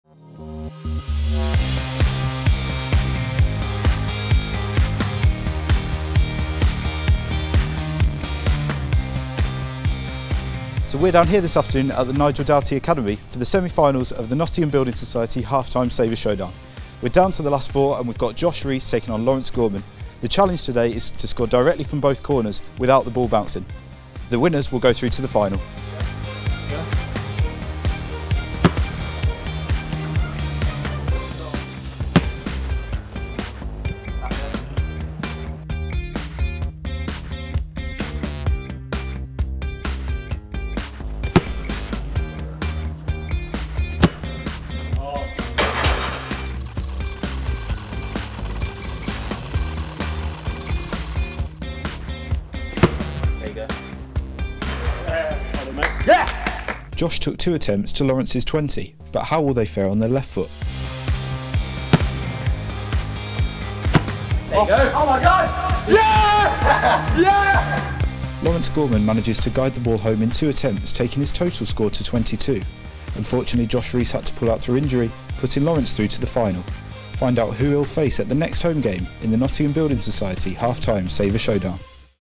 Half Time Saver Showdown: Semi Final 1
The Semi Finals of the Nottingham Building Society Half Time Saver Showdown sees the semi-finalists taking on the corner challenge. Featuring Josh Rees and Lawrence Gorman.